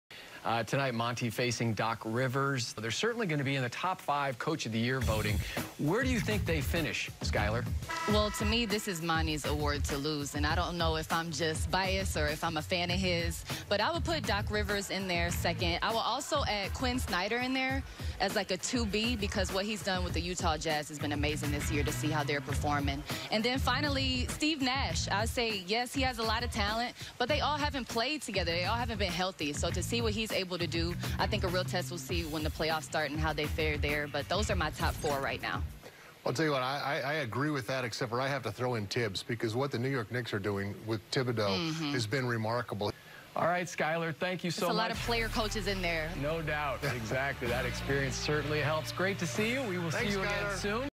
Who is the NBA's Coach of the Year? Skylar Diggins-Smith on Suns Live!
Skylar Diggins-Smith gives some of her top choices for the coach of the year award, including Suns' Monty Williams, 76ers' Doc Rivers, Jazz's Quin Snyder, Nets' Steve Nash, etc.